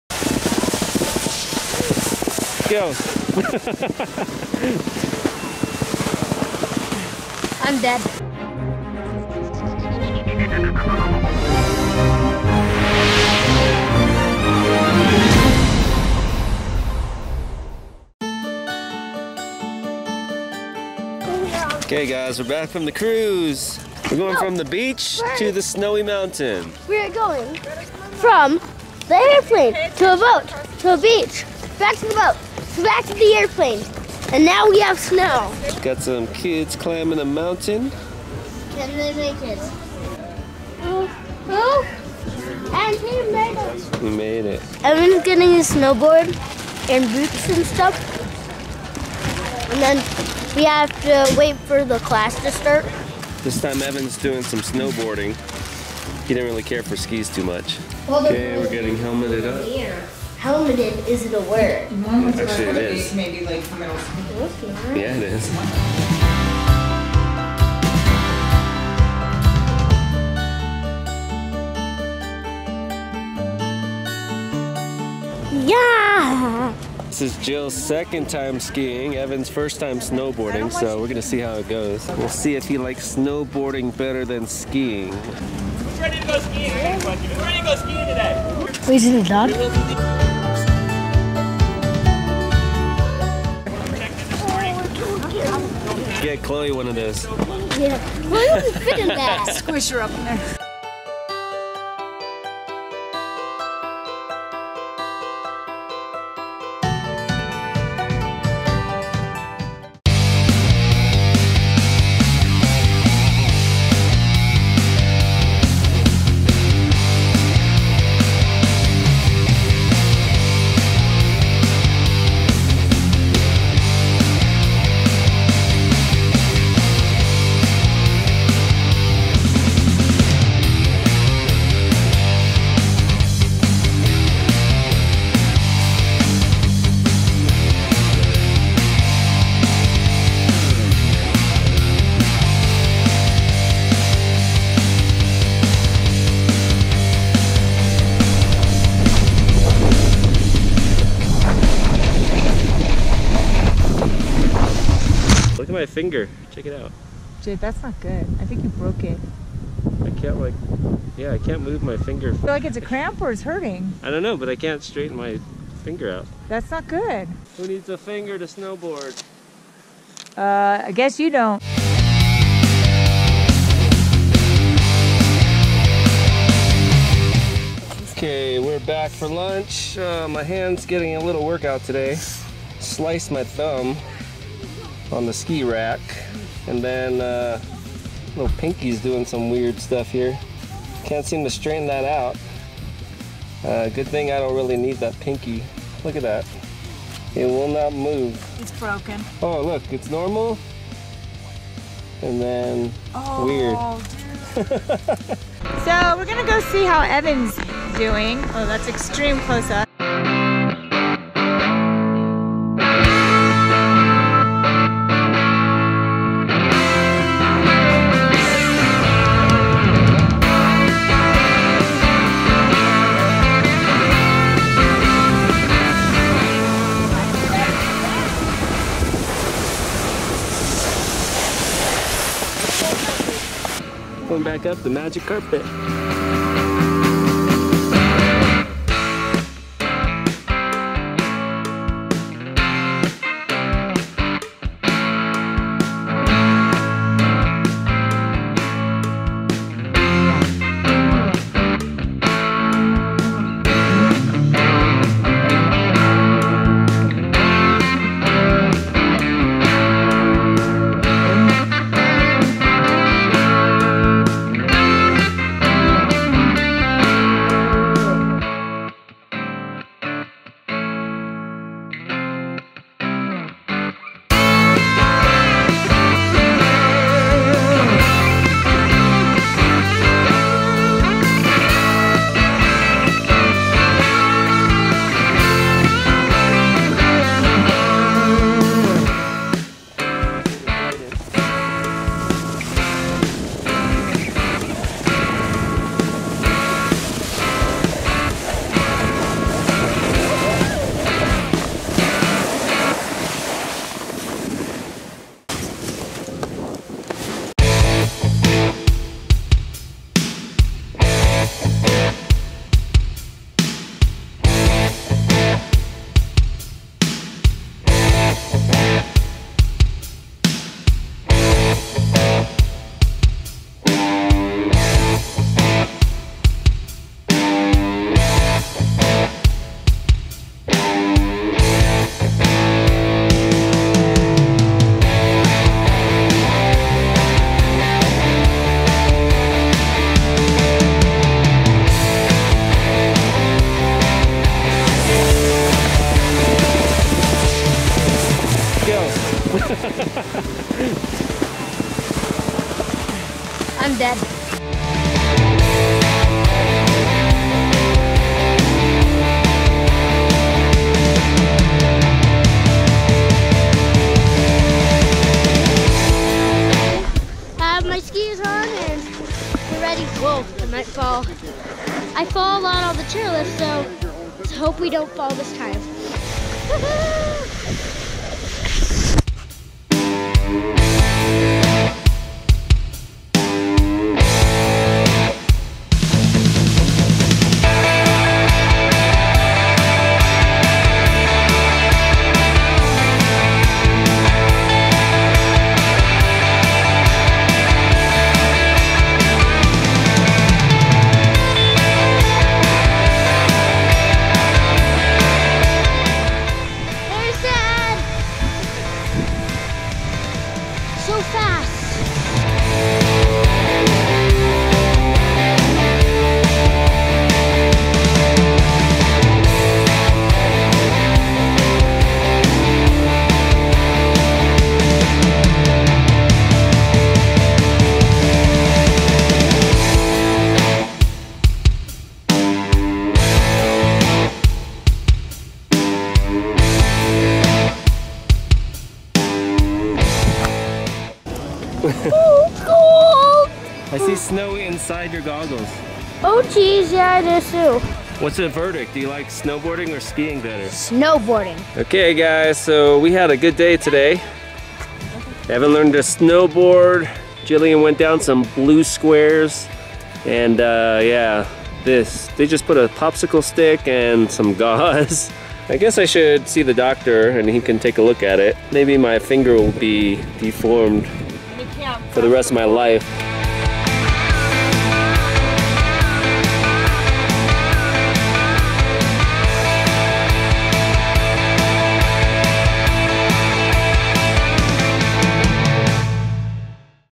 BROKEN FINGER!!! Snowboarding Gone Wrong!
We got back from our Caribbean cruise vacation and heard there was still snow on the mountain.  So we're heading up for more fun on the slopes!  This time around, Evan decided to try snowboarding.  We all left in one piece, but Dad had to take a trip to Dr.'s office after his finger came out looking a little funky.